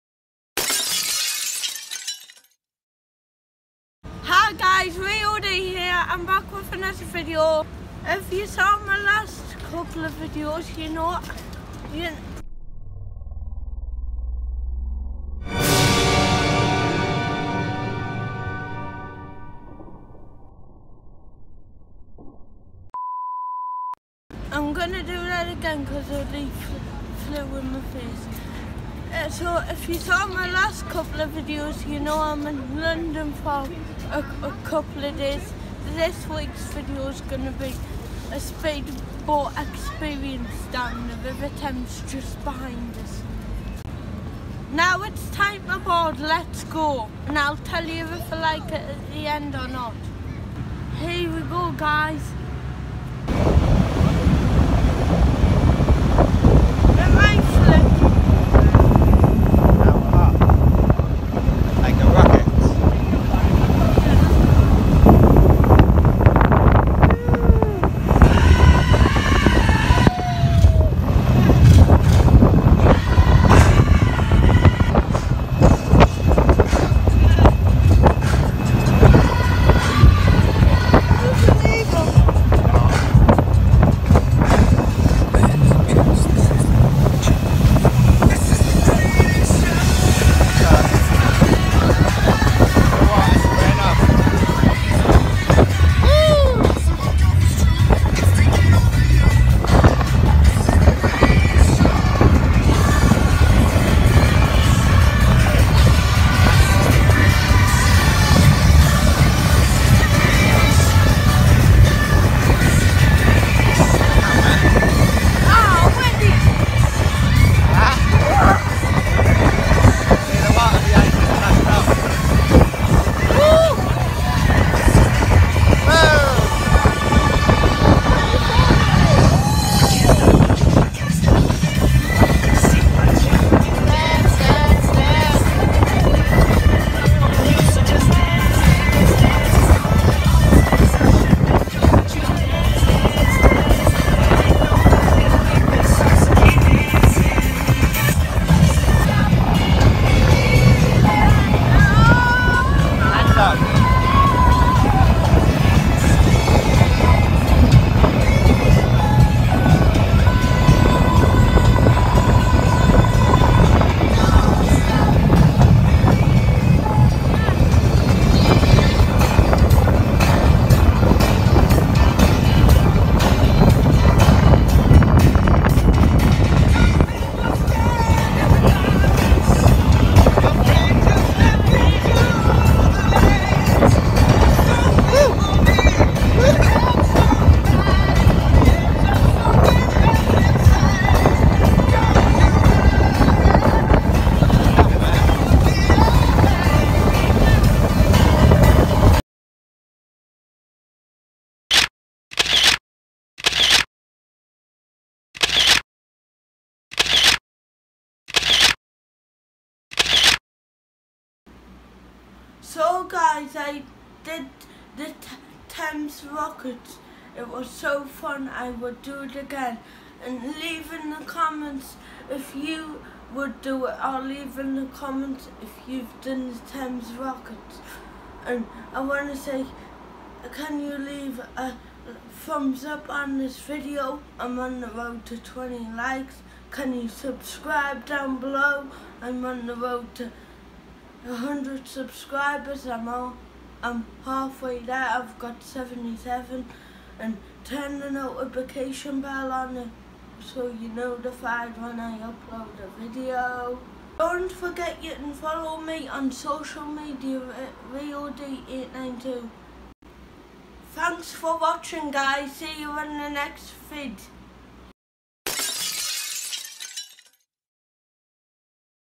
Thames rockets speed boat ride
Speed boat ride down the river Thames in London. Slow n steady until you get under tower bridge then you hear the engines start to roar and feel the wind in your hair as you reach the part of the river with no speed limit!

Weaving our way down the river Thames at 30 knots (35mph) all the way down past Canary Wharf and then back again.

Once you hit tower bridge on the way back they slow it down and you get to see the sights with the guide telling you all about them.

How many sights can you spot? Can you name the sights in my photos?

Great family fun for all ages and I’d recommend this boat trip if you visit London